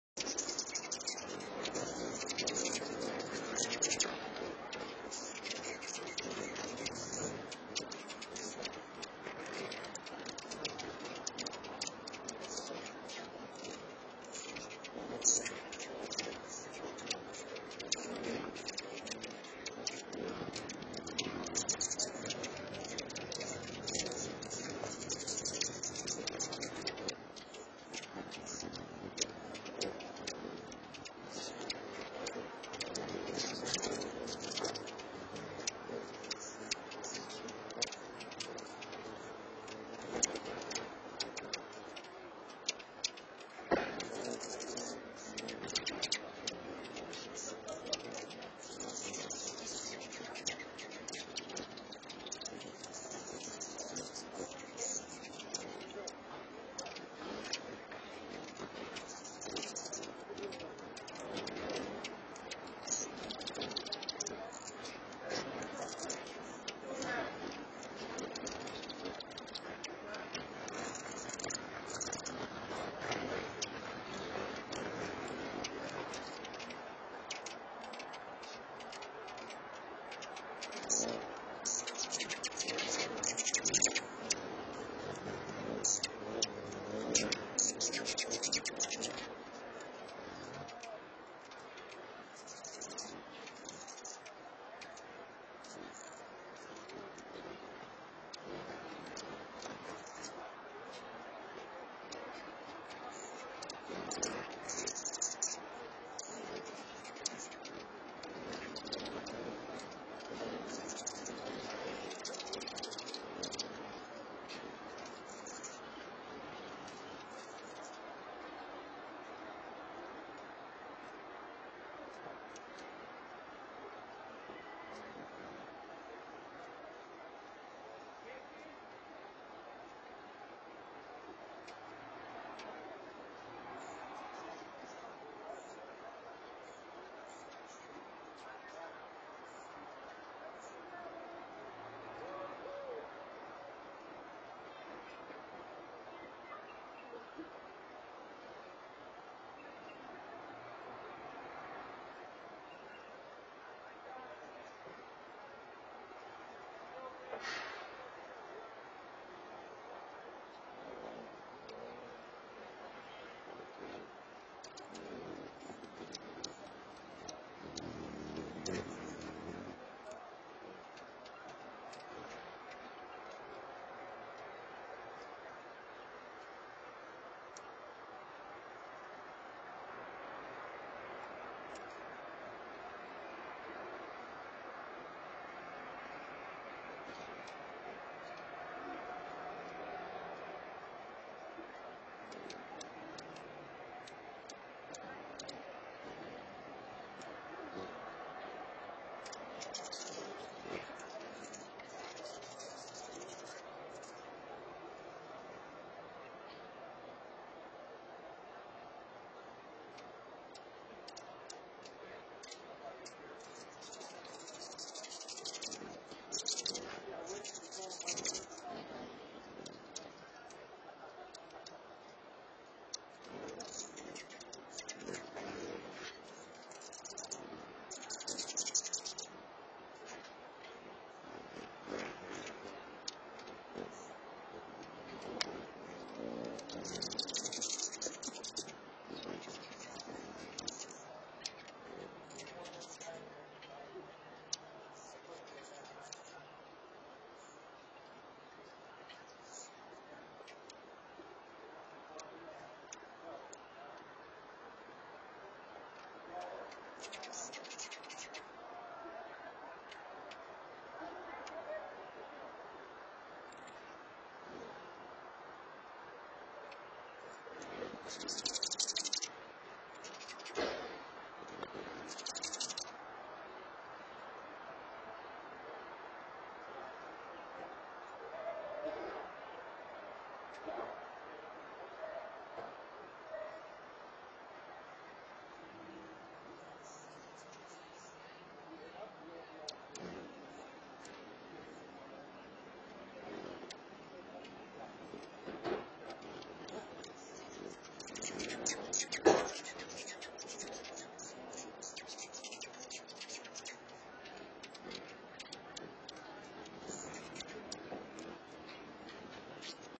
The Rock Cracks Again
At approx 10:34am (45 seconds into the video) you can hear a good "bang!" and the crew on the diving platform move quickly to the safe side of the rock.